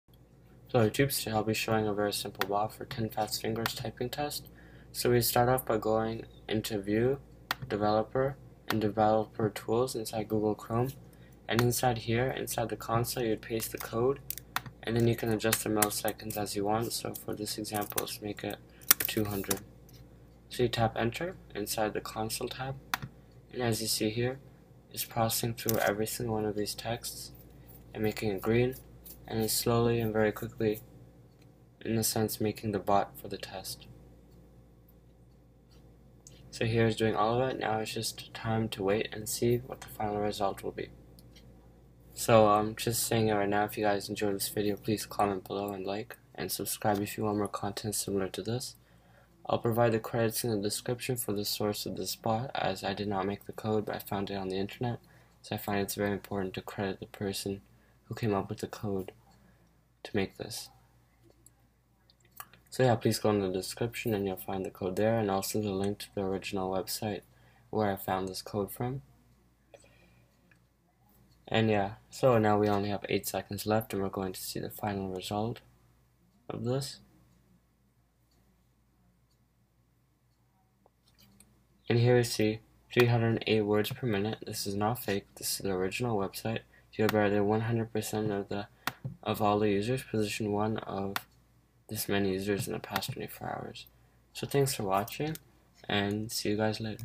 How to: Cheat 10FastFingers - EASY Bot! [Working 2016] - Top Score in the World!
Very easy bot for 10FastFingers, simply plop the code in the Console as shown from this website. Credits to the guy for creating this: